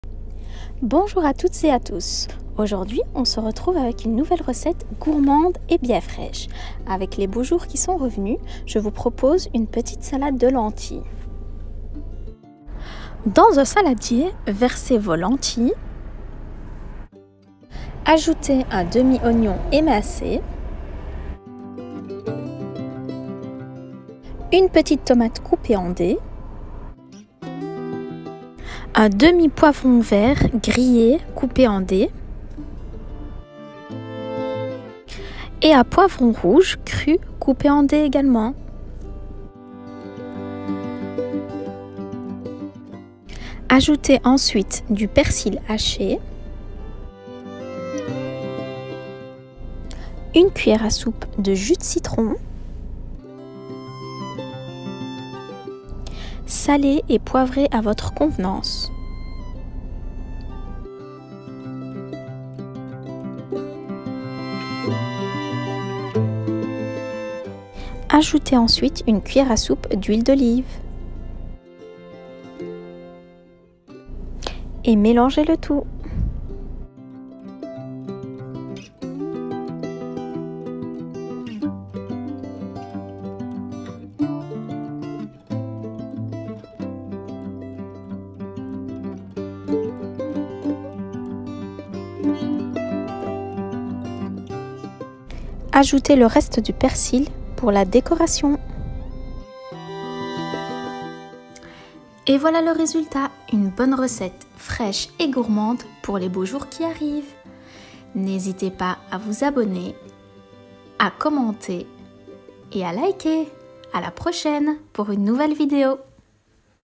La Célèbre Salade Fraîche aux Lentilles simple ,rapide ,facile à faire .recette marocaine.
Voici la recette de la Salade Fraîche aux Lentilles

Ingrédients:

+- 200g de Lentille Verte
Un demi oignon
Une petite tomates
Un demi poivron grillé vert
Un demi poivron cru rouge
Du persil
Une cuillère à soupe de jus de citron
Une cuillère à soupe d’huile d’olive
Sel/Poivre à votre convenance

C’est Gratuit! **